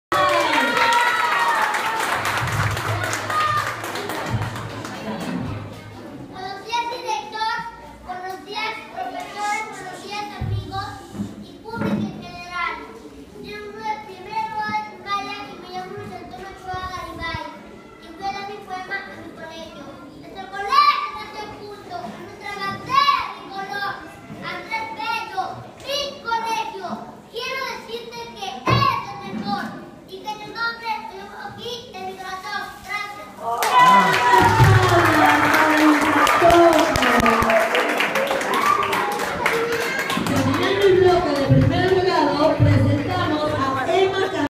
Toñito Ochoa recitando en Andrés Bello
Aquí Luis Antonio Ochoa Garibay en el día de la palabra en el Colegio Andrés Bello